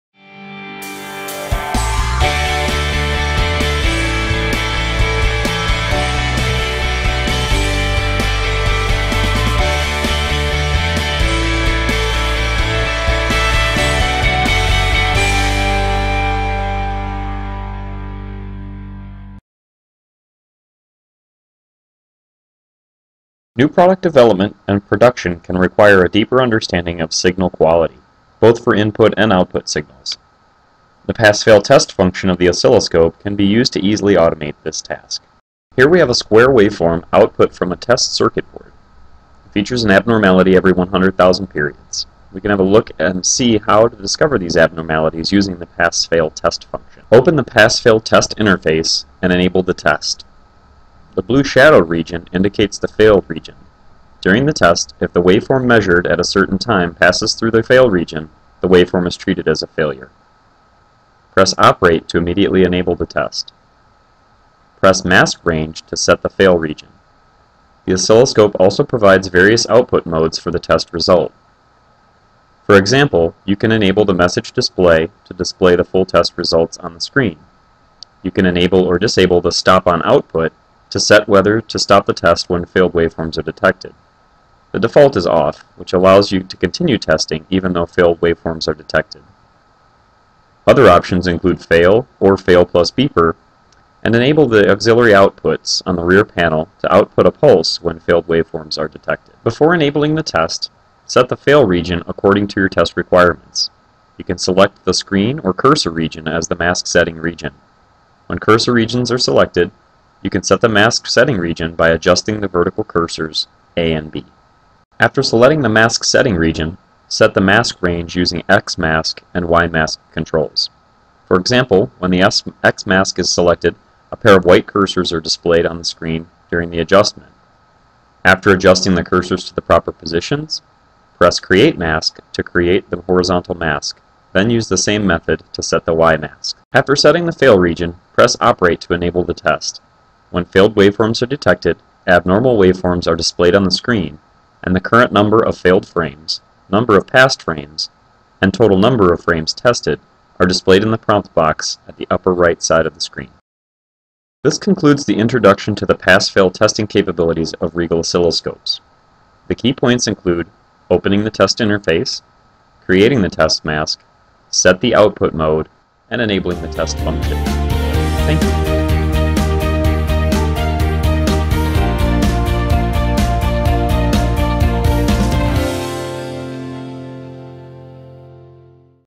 Using Pass/Fail Analysis
Using a easily defined mask the oscilloscope can perform pass fail tests to quickly identify out of bounds conditions on a system under test.